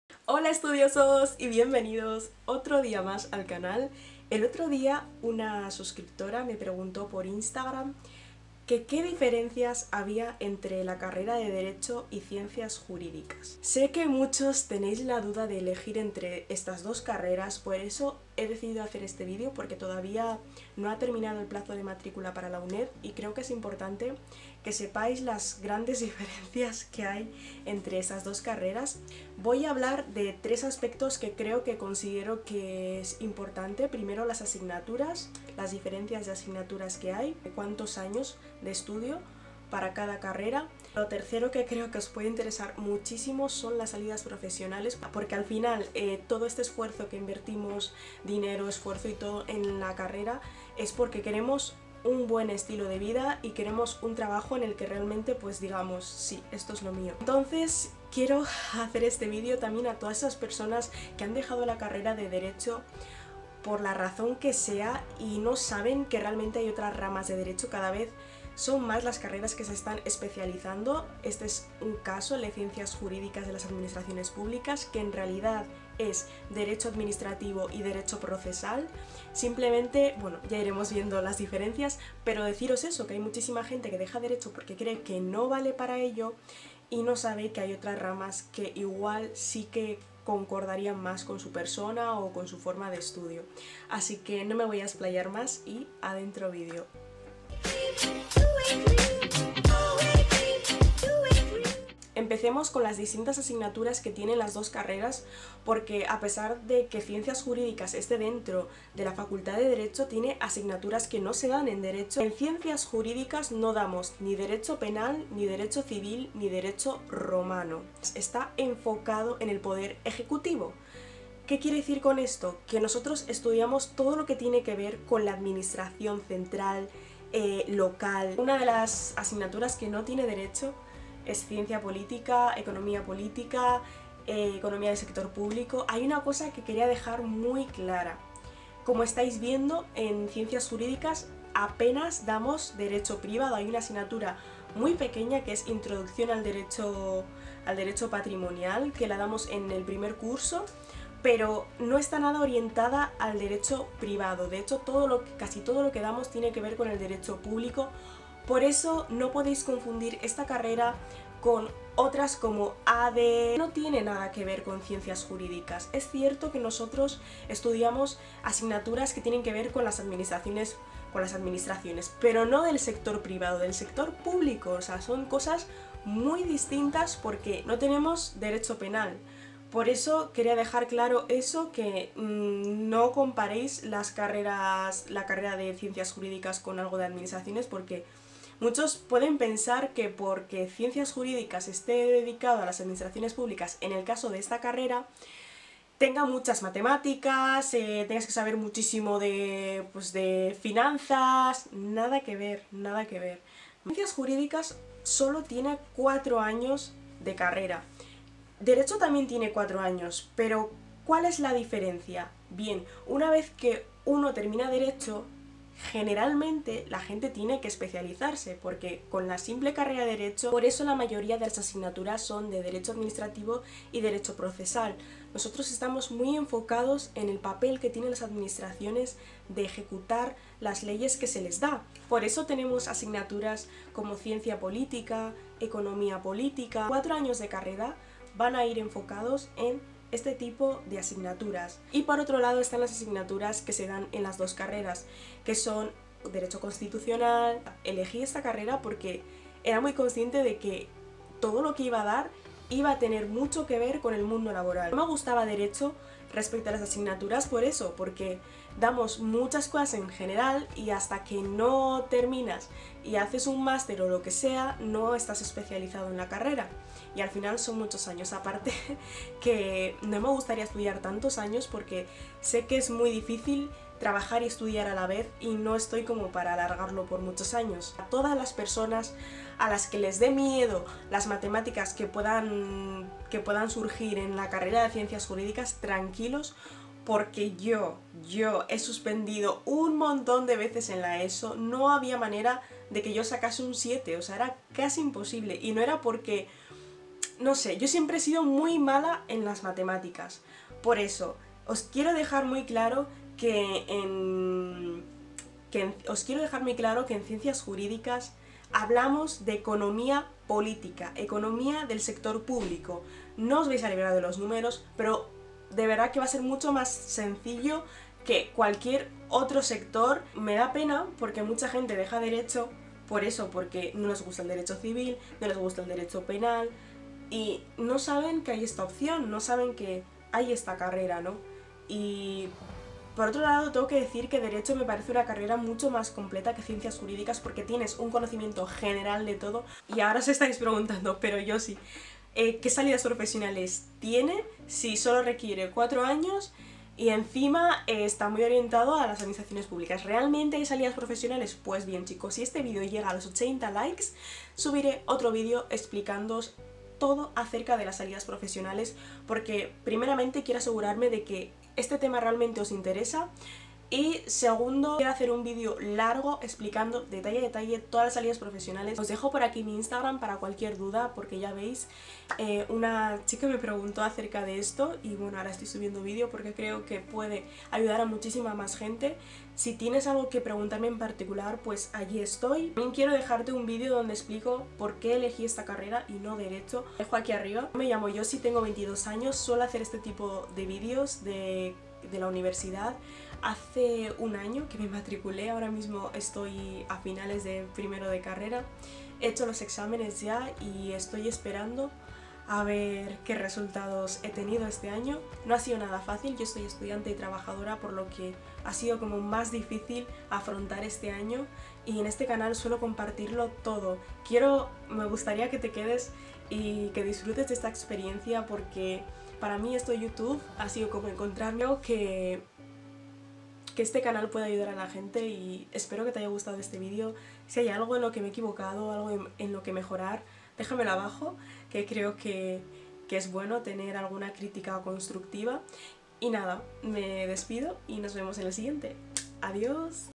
⚖DIFERENCIAS GRADO DERECHO Y CIENCIAS JURÍDICAS ⚖| JosieGoms
¡Es un placer para mí poder compartir toda mi experiencia en la UNED, en la facultad de DERECHO , carrera "CIENCIAS JURÍDICAS DE LAS ADMINISTRACIONES PÚBLICAS"

📽 GRABACIÓN📽

●MI RUTINA DE ESTUDIO PARA DERECHO

●¿CUANTO CUESTA LA CARRERA A DISTANCIA?

-Biblioteca Youtube